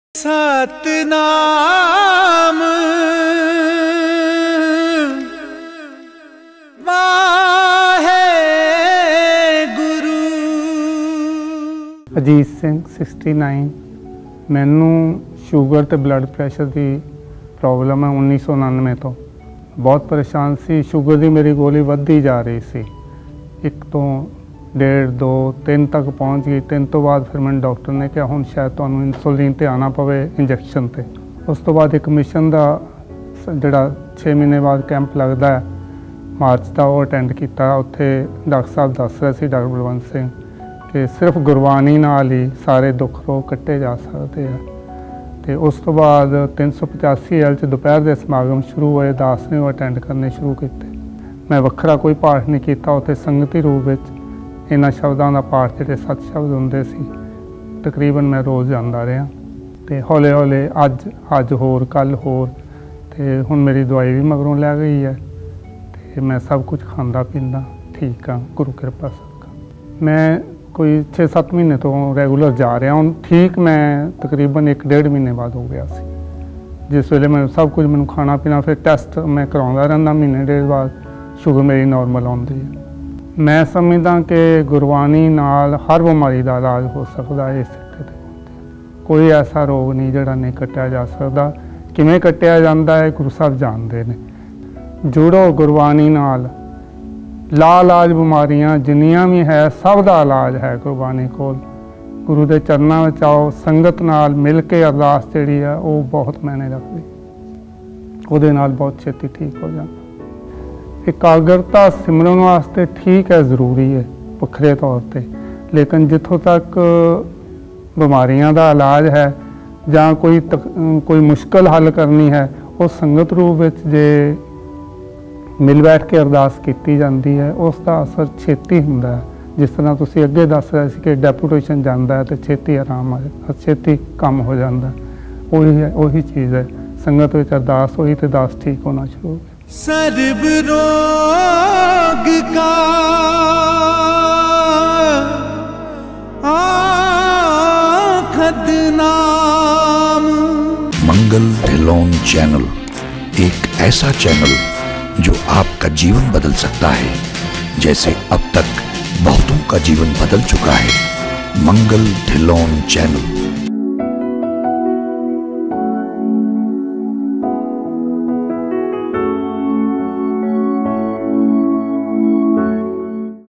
684.(70) Gurubani Ke Chamatkar Part-70 (Miracle of Gurubani Ajit Singh Case) -Mangal Dhillon
684.(70) Gurubani Ke Chamatkar Part-70 (Miracle of Gurubani Ajit Singh's Sugar & High Blood Pressure Case) -Mangal Dhillon